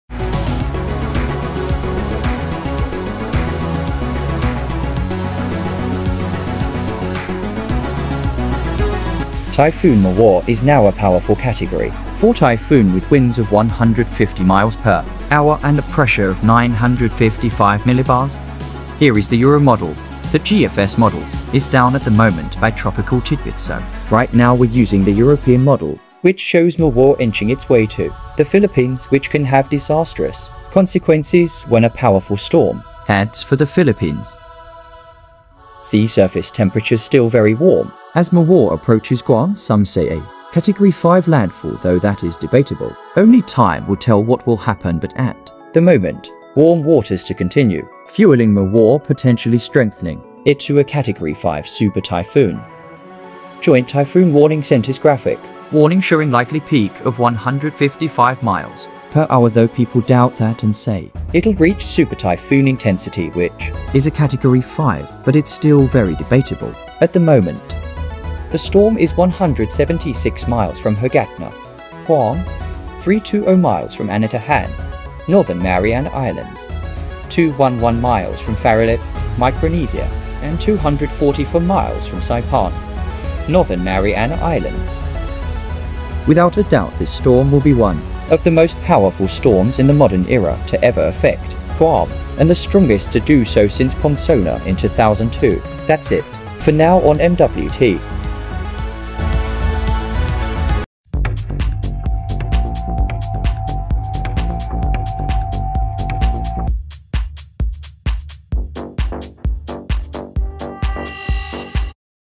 Typhoon Mawar a very Powerful Category 4 Setting its Sights on Guam | Update 4
A low-pressure area south-southwest of Chuuk Islands developed into a tropical depression on May 19.[13][14] On May 20, it was later designated as 02W by the JTWC and named the storm Mawar a few hours later by the JMA before it intensified into a severe tropical storm on the next day. As it entered more favorable conditions and low wind shear, it was upgraded into Category 1-equivalent typhoon by the JTWC. The JMA followed suit and upgraded Mawar into a typhoon. The storm was further upgraded to Category 2-equivlent status by the JTWC on May 22. It later reached Category 3-equivalent status on the same day. On the same day, the PAGASA forecasted that Mawar is likely to develop into a super typhoon. A couple of hours later, the JTWC updated Mawar into a Category 4-equivalent typhoon while JMA keeping it as a very strong typhoon status